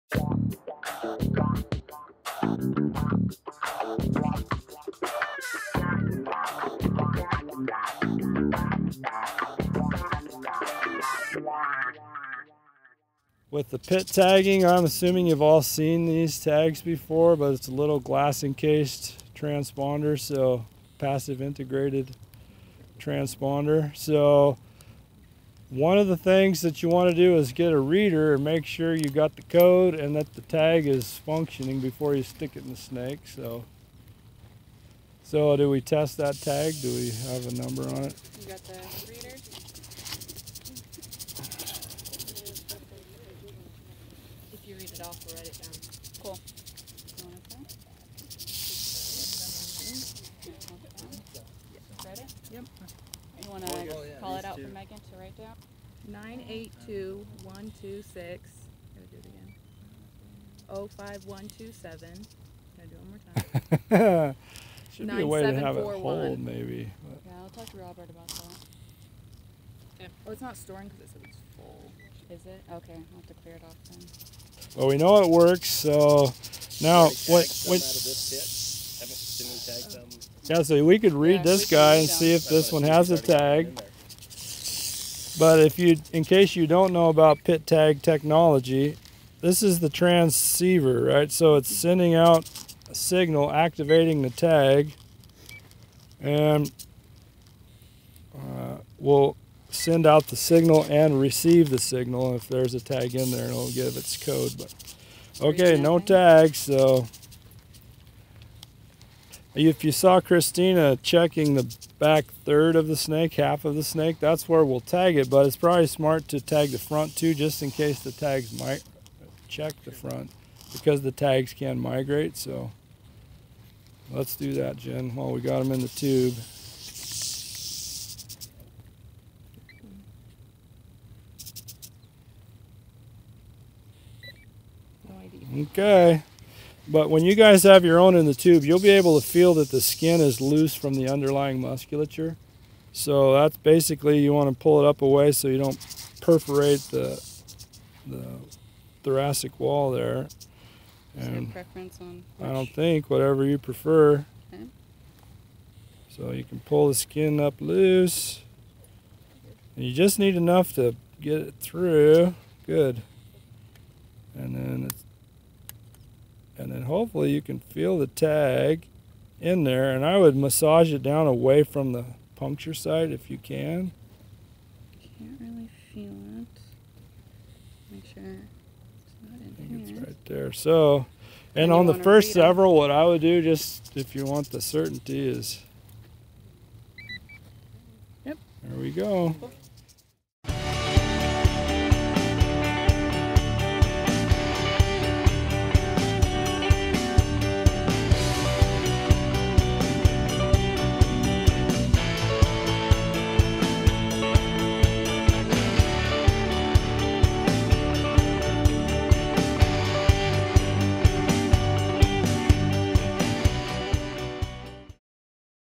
PIT Tagging A Rattlesnake
Dr. John Cossel of Northwest Nazarene University and Kristina Parker from Boise State University demonstrate how to insert a passive integrated transponder (PIT) tag into a rattlesnake.  These tags allow researchers to track movements of individual reptiles to provide information about population sizes and home ranges.  This training was part of a Snake Safety and Handling course offered to researchers at the Orchard Combat Training Center, Idaho, in May 2018.